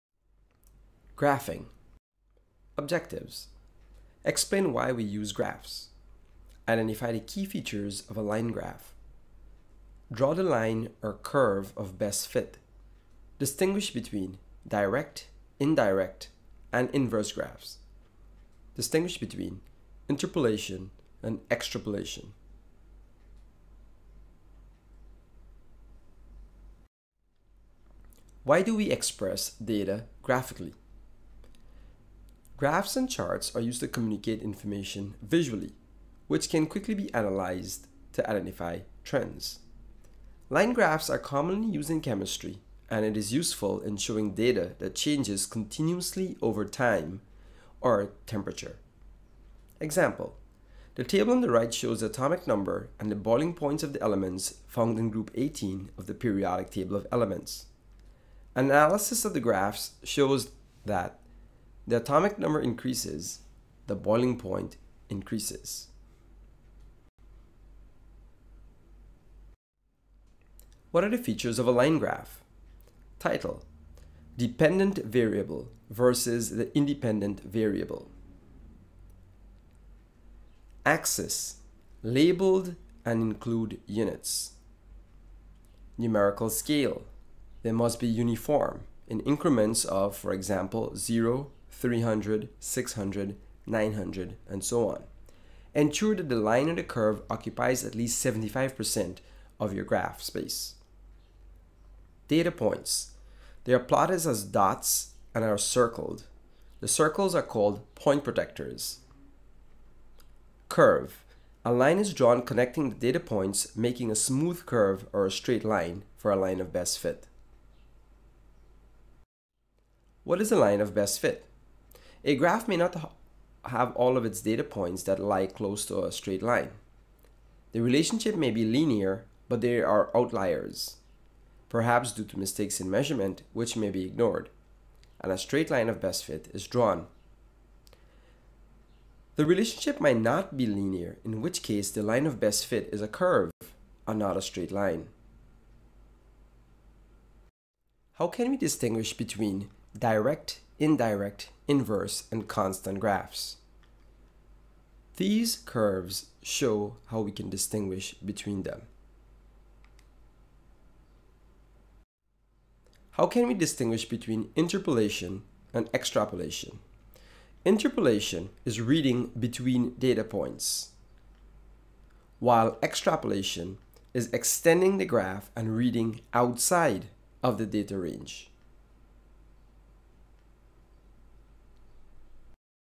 110 Graphing
Explain why we use graphs
Identify the key features of a line graph
Draw the line or curve of best fit
Distinguish between direct, indirect, and inverse graphs
Distinguish between interpolation and extrapolation